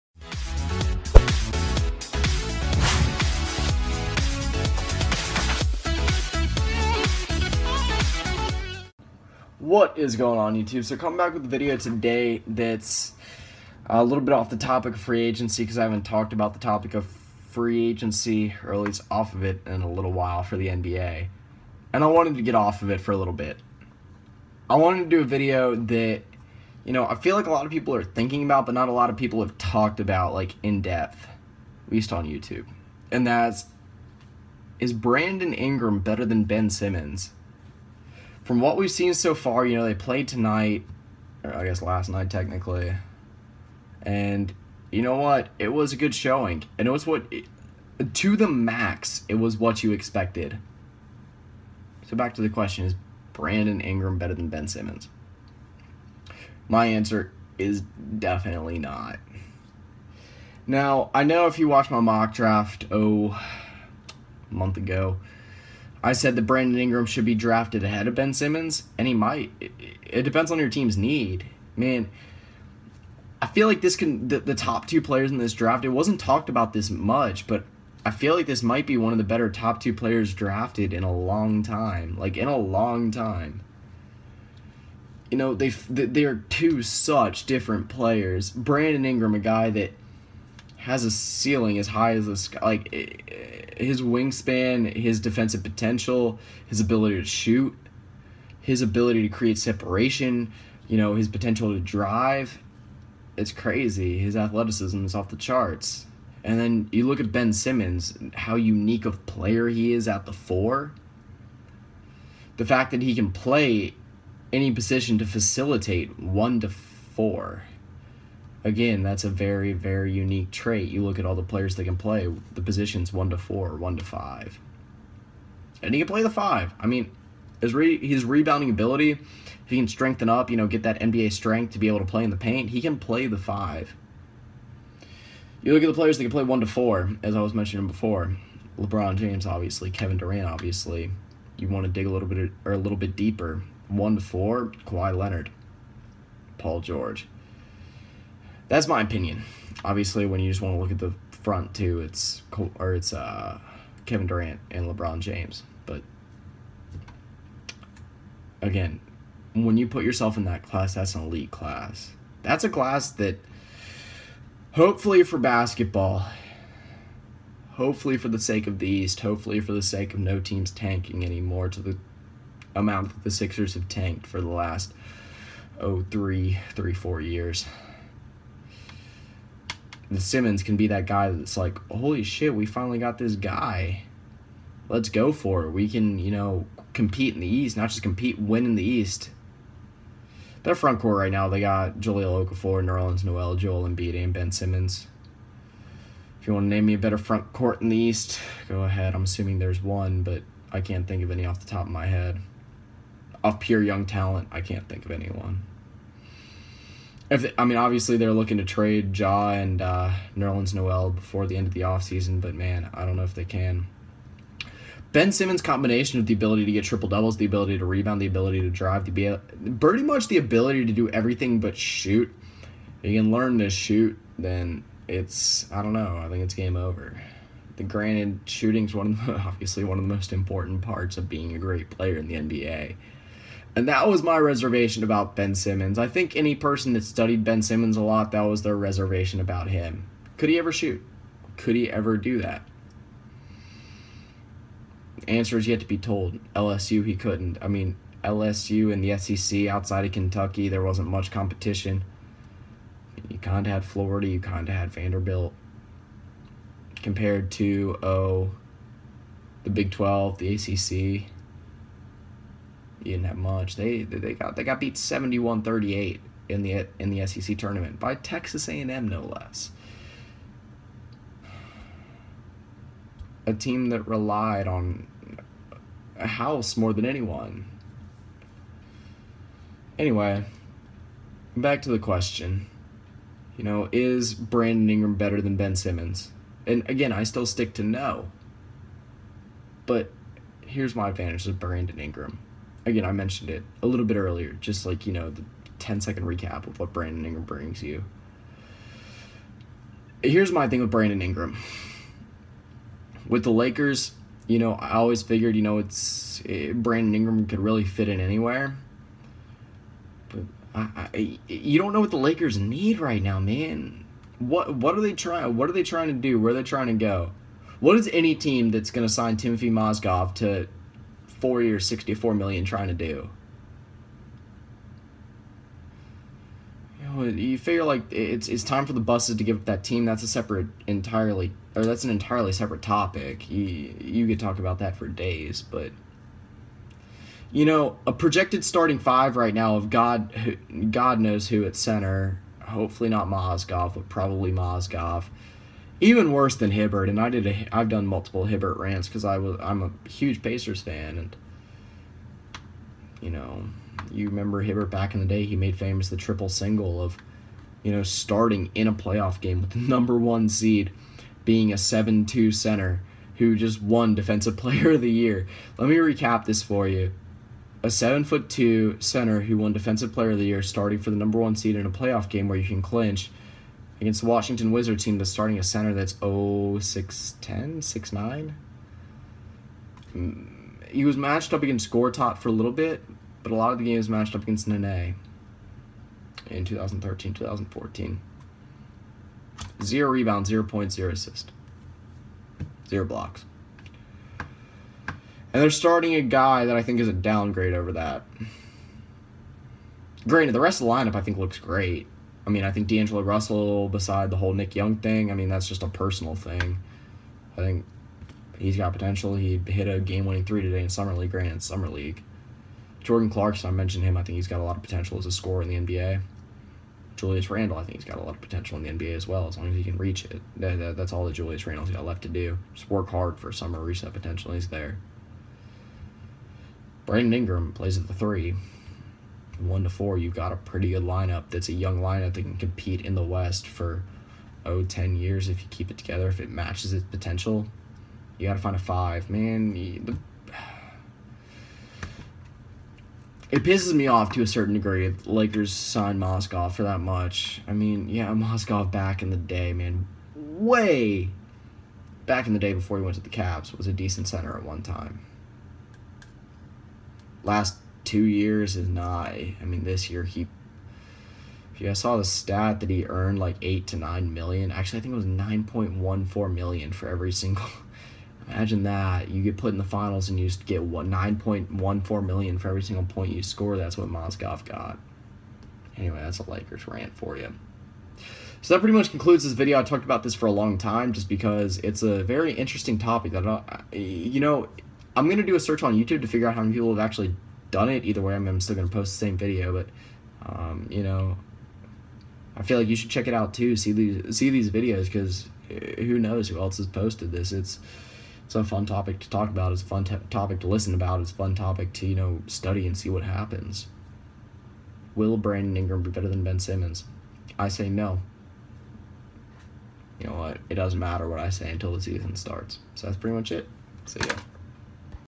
My insight on who will be better? Ben Simmons or Brandon Ingram?!
My insight on who will be better between the two top picks in the 2016 NBA Draft between Ben Simmons and Brandon Ingram. In this video I talk about the angles for which each player is better than the other.